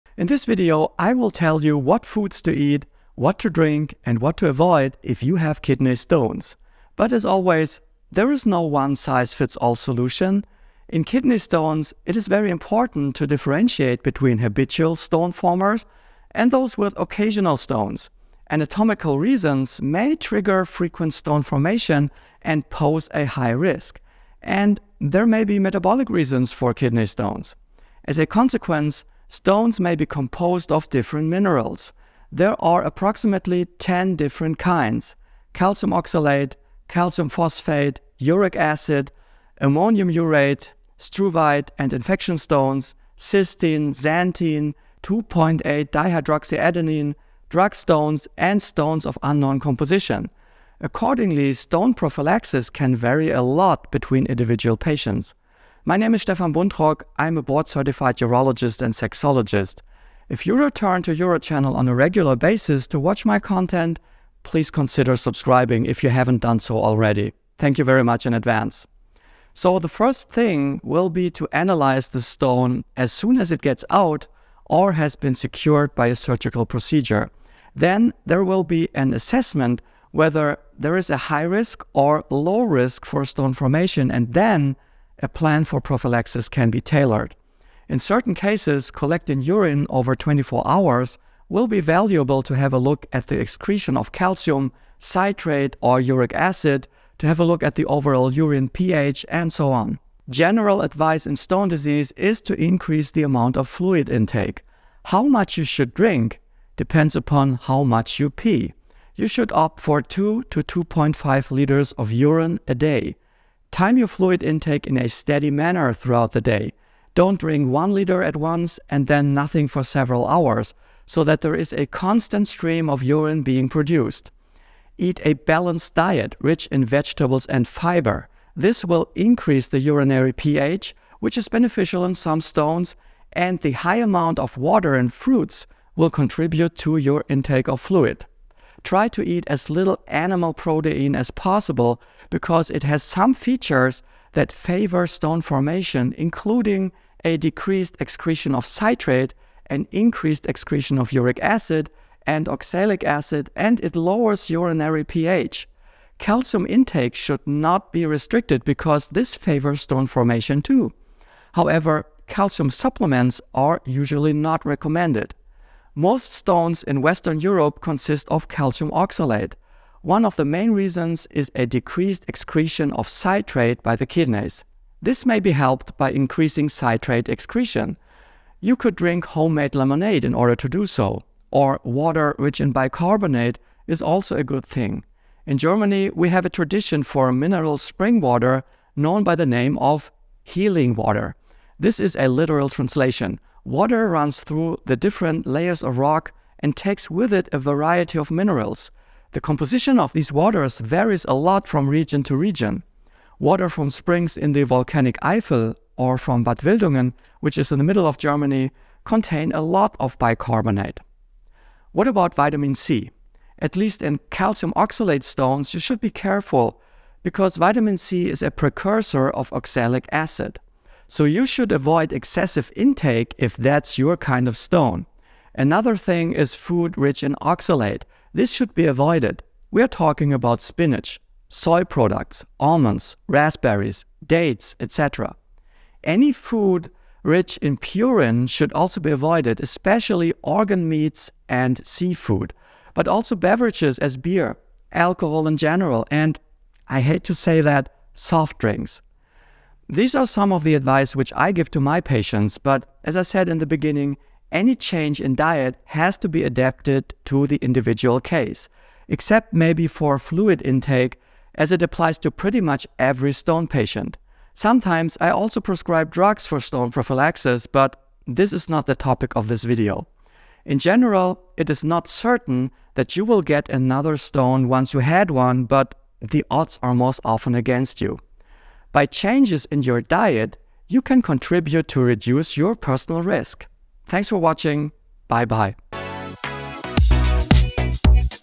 Avoid kidney stones with certain foods? | UroChannel
In this video on UroChannel, urologist and sexologist from Göttingen, Dr. med. Dr. phil.  Stefan Buntrock provides valuable information on managing kidney stones through diet and lifestyle choices. He emphasizes that there is no one-size-fits-all solution when it comes to kidney stones, as the causes and composition of stones can vary among individuals. He distinguishes between habitual stone formers and those with occasional stones, highlighting the importance of understanding the underlying anatomical and metabolic reasons for stone formation. Buntrock advises analyzing the stone composition and assessing the risk level before tailoring a personalized plan for stone prophylaxis. He recommends increasing fluid intake to maintain a steady stream of urine production, aiming for 2 to 2.5 liters of urine per day. A balanced diet rich in vegetables and fiber is encouraged, as it can increase urinary pH and fluid intake. Animal protein should be limited, as it can contribute to stone formation, while calcium intake should not be restricted. Buntrock suggests increasing citrate excretion through home-made lemonade or bicarbonate-rich mineral spring water. Foods and beverages high in oxalate and purine, such as spinach, soy products, organ meats, seafood, beer, alcohol, and soft drinks, should be avoided. Buntrock concludes by emphasizing that diet changes should be adapted to individual cases and that reducing personal risk through dietary modifications can help prevent future stone formation.